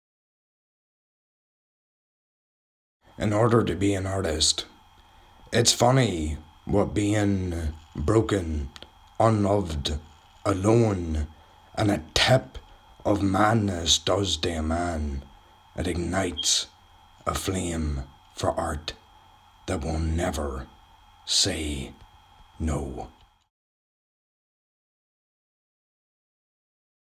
ISLAND Arts' Poem of the Day, 'In Order to be an Artist' by Paul Butterfield
If you would like to contribute to ISLAND Arts' 'Poem of the Day,' and appear on @Lisburns98 FM and @BangorFM radio broadcasts.